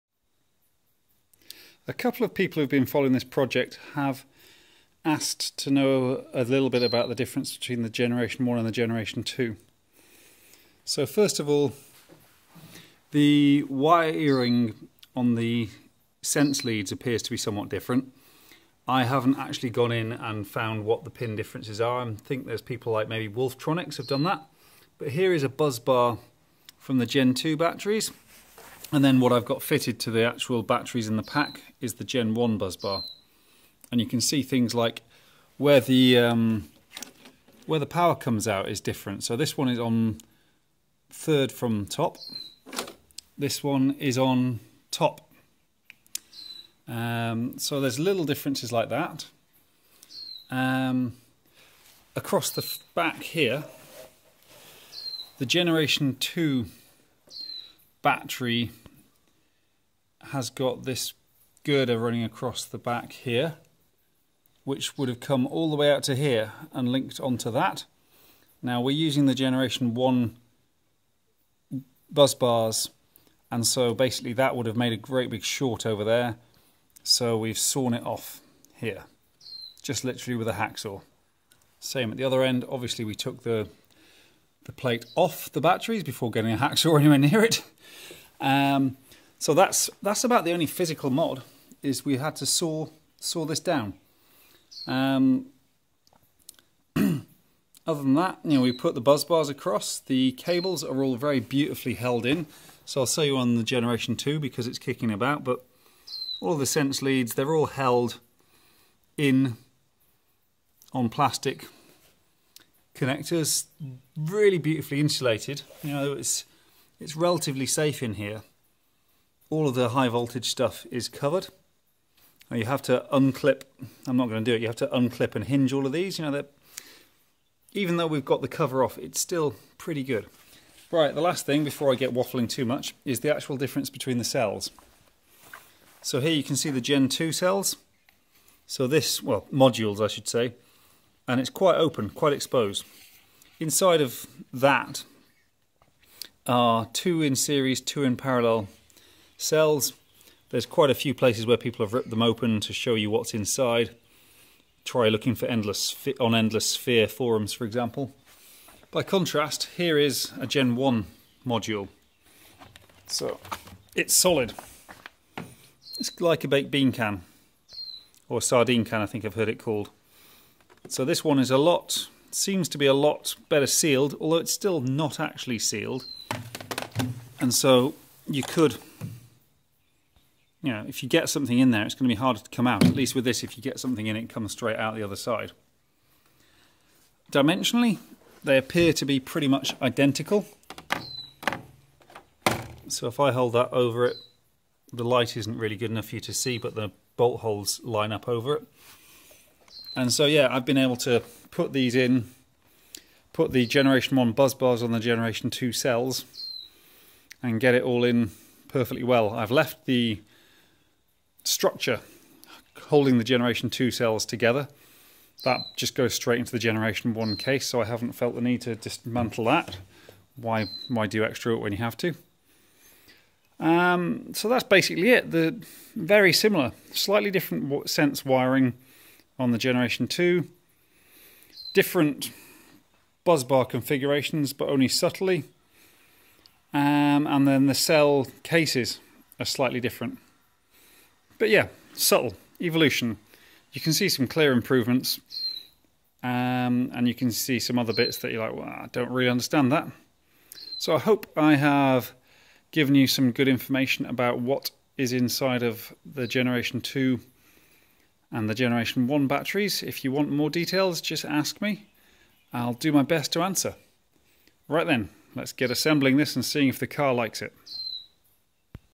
Nissan Leaf G2 G1 cells comparison in G1 case
What's it like inside the battery of a Nissan Leaf?  Here I show you while also explaining some of the differences between Generation 1 and Generation 2 modules and a bit about what you need to do to make the 2 fit in the 1.
Clarification:
G1 is roughly 2012 and older 24kWh.
G2 is roughly 2014 and newer, 24kWh with slightly better life expectancy.
The noise in the background is a silvereye, a really nice little NZ native bird. For some reason it felt the need to contribute.